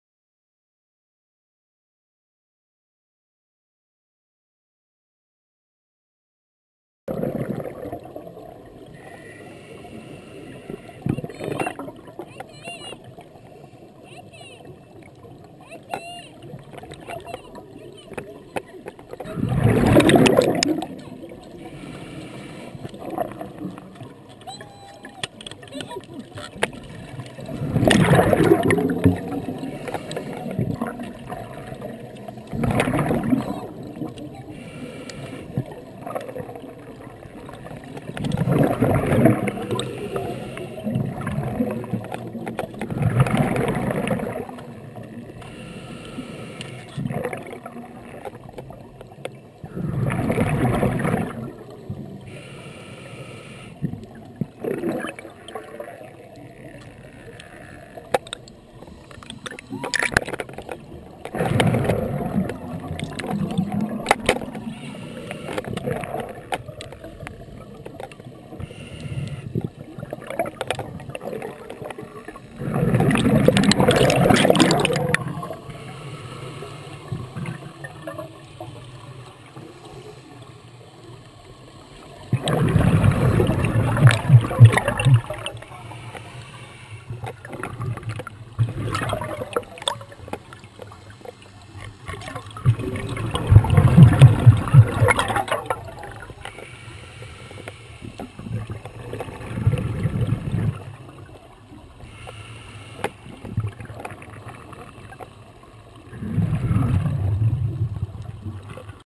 Lisa and the Sea Urchins
Lisa is now 3 days into diving and her comfort level is high.  She is starting to spot things on her own and she sees 2 Sea Urchins and is very excited.  This is why we dive never forget it.